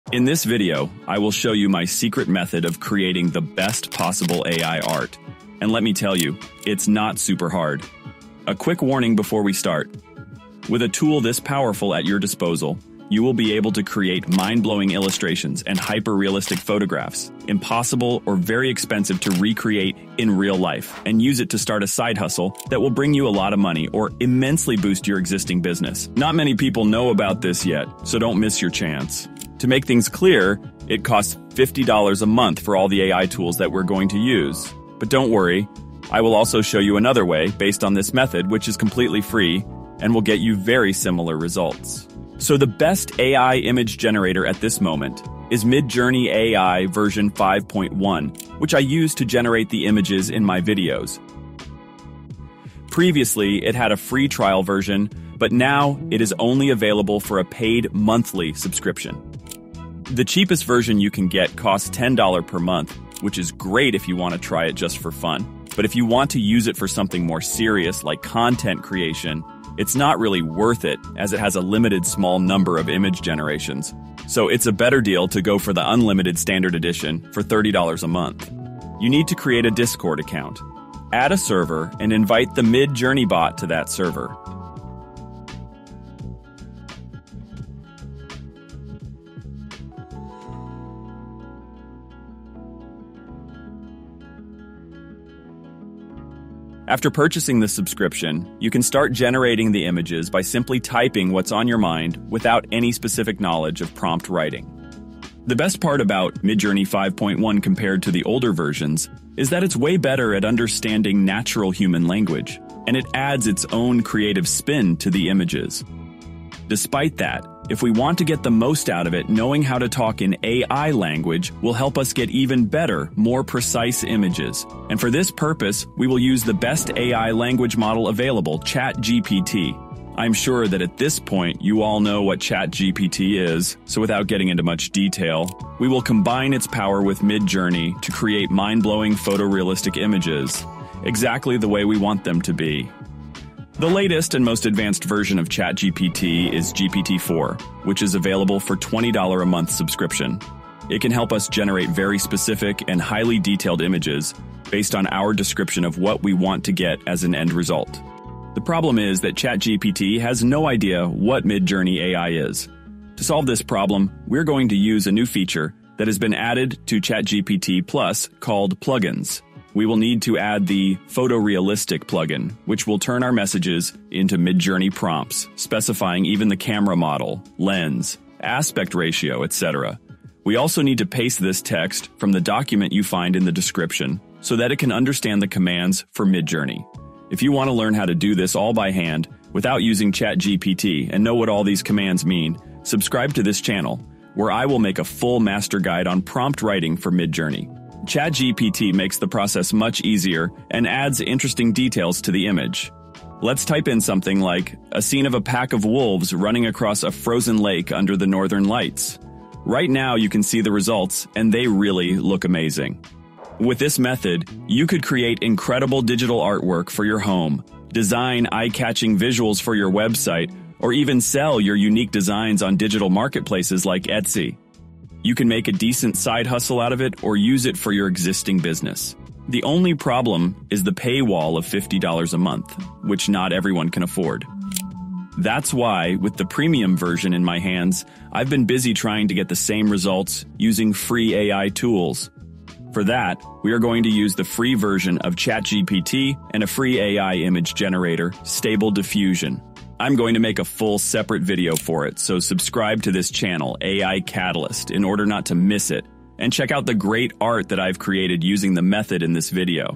Secret Method of Creating THE BEST AI Art
In this video I will show you how to use Midjourney v5.1 and ChatGPT 4 to create the best quality possible AI Art 🤯